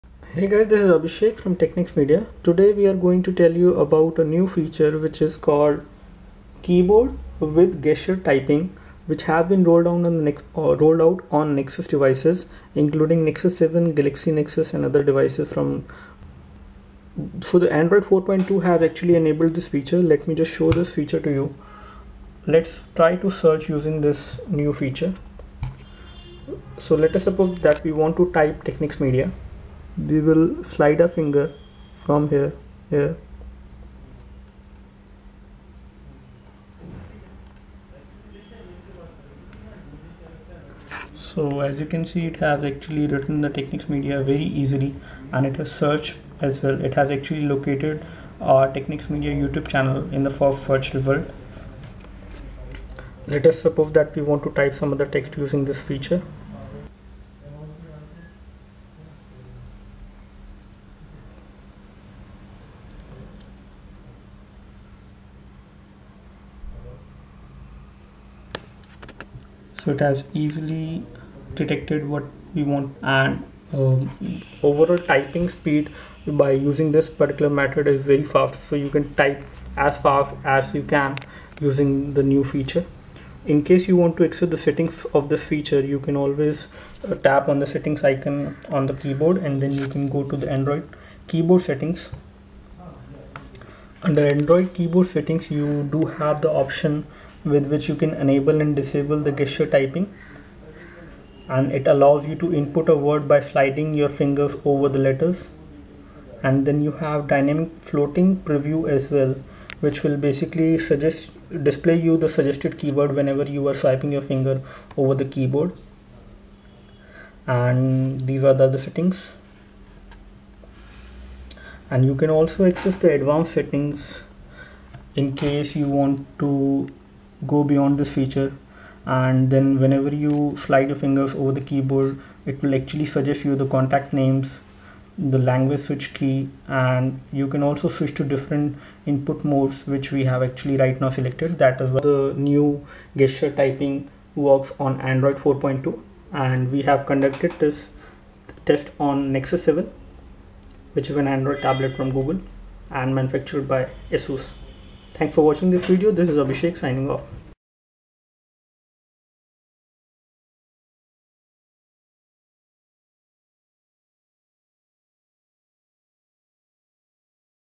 Gesture Typing On Android 4.2 Finger Swipe on On Screen Keyboard
This video tells you about new gesture typing feature rolled out in android with 4.2 on tablets and phones.